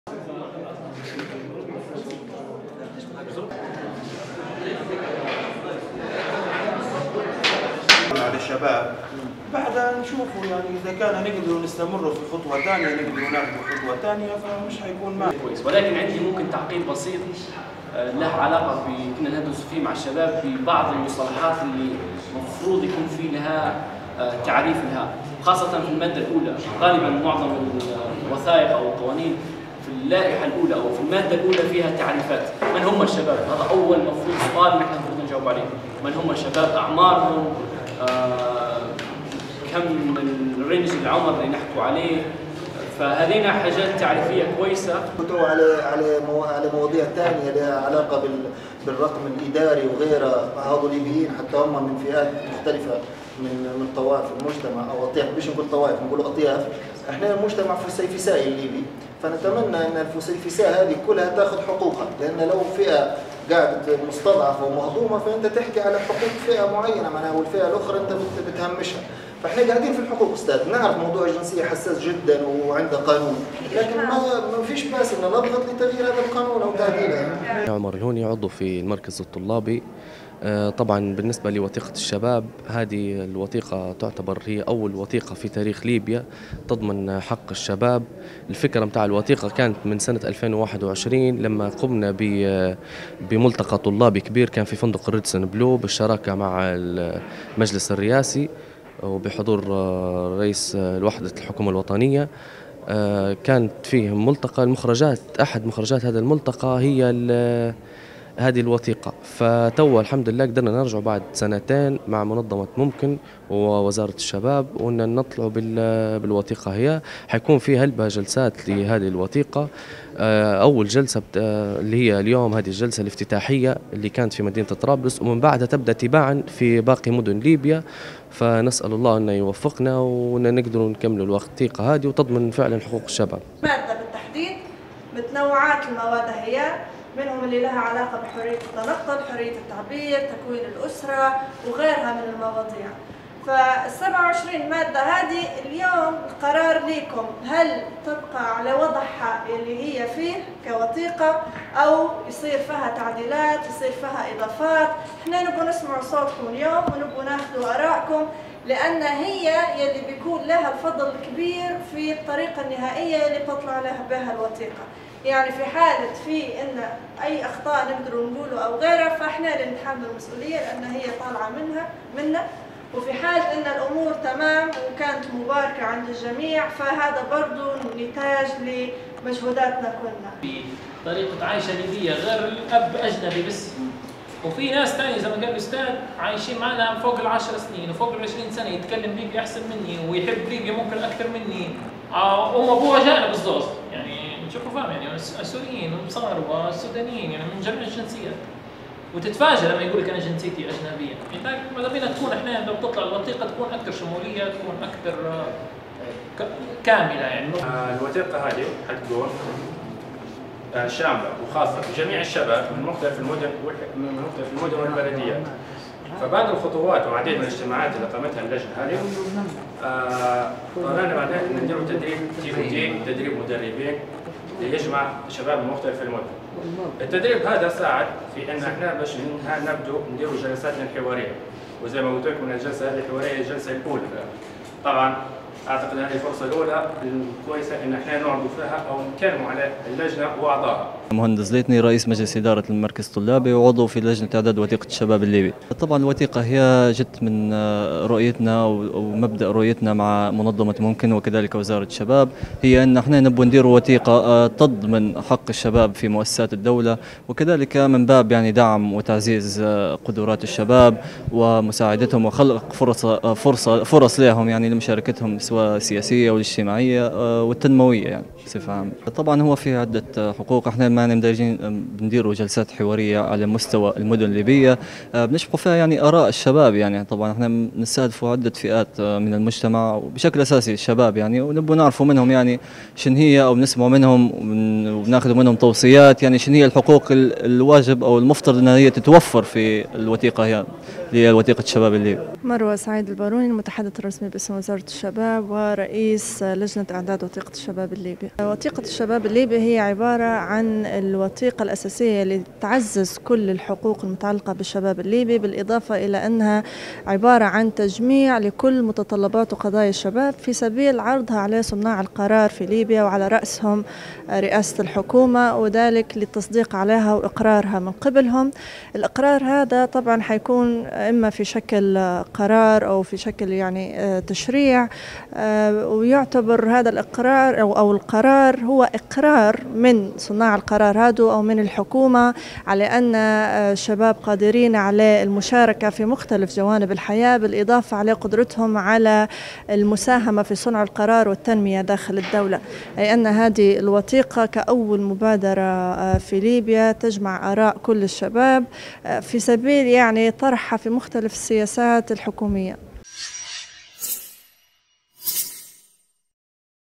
إعداد مسودة وثيقة الشباب الليبي
في إطار العمل على إعداد مسودة وثيقة الشباب الليبي وضمن سلسلة الجلسات الحوارية التي ستقام في مختلف المدن الليبية تباعًا.
أقيمت مساء اليوم الأحد ببيت نويجي للثقافة في المدينة القديمة، الجلسة الحوارية الأولى في مدينة طرابلس ضمن سلسلة الحواريات التي يعمل فيها الشباب على مسودة الوثيقة من تنظيم منظمة ممكن للتوعية والإعلام وبمشاركة وزارة الشباب.
 حيث شارك الحضور آرائهم وتصوّراتهم عن مايجب أن تتضمنه الوثيقة التي تجسّد تطلعات شريحة الشباب الليبي، بما يضمن الحقوق والواجبات الرئيسية للشباب، تمهيدا للتصديق عليها وإقرارها من قبل صناع القرار.